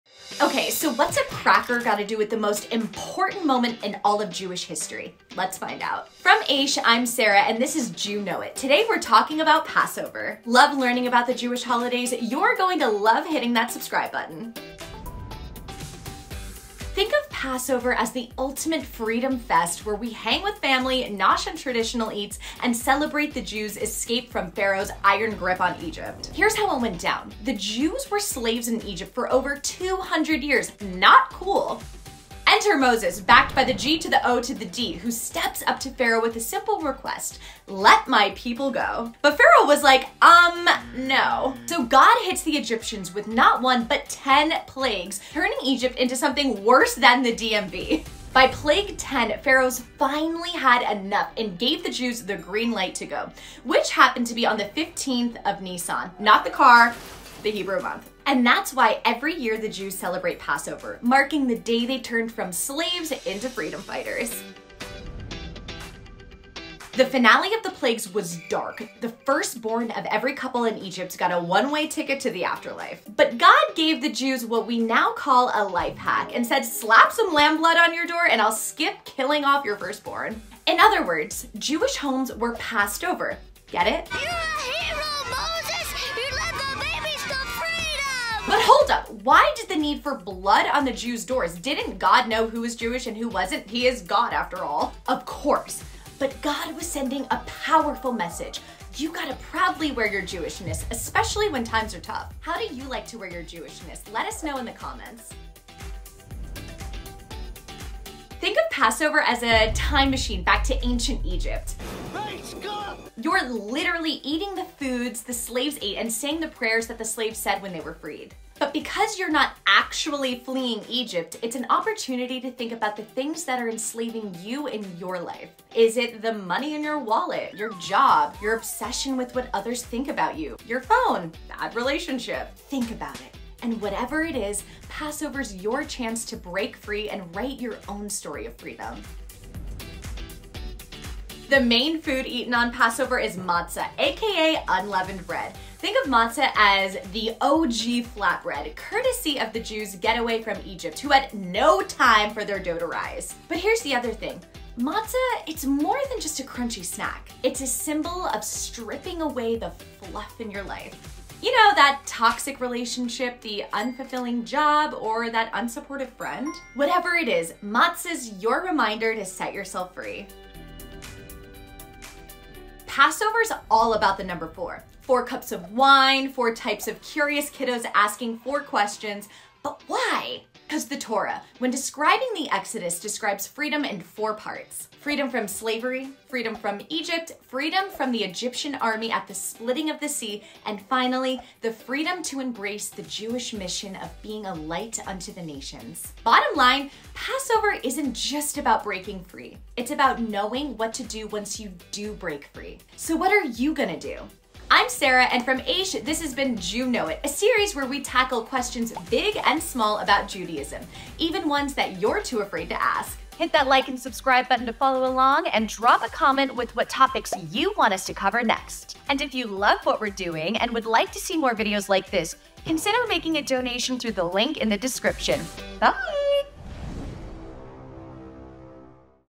From Slavery to Freedom: Your Passover Crash Course | Jew Know It
Discover the rich history of Passover, a holiday celebrating freedom. Learn about traditional Passover foods, the significance of the number four, and other Passover customs.

👉🏼 Connect and follow for more Jewish Wisdom 💫